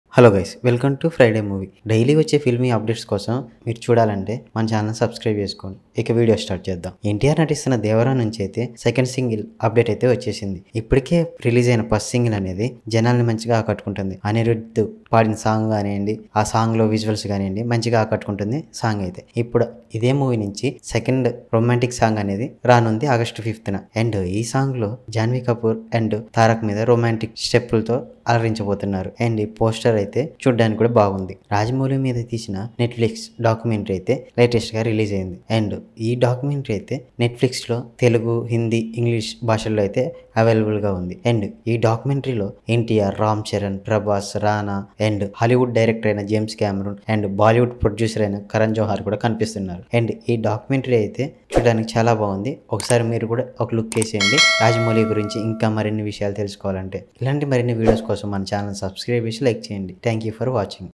Devara Second Song Update | Ss Rajamouli, Netflix Documentary | NTR, Janvi Kapoor
Devara Second Song Update | Ss Rajamouli, Documentary | NTR, Janvi Kapoor

'Devara Part 1': Makers announce release date of second single with a romantic poster of Jr NTR and Janhvi Kapoor - See inside. Jr NTR and Janhvi Kapoor's 'Devara Part 1' is set to release on September 27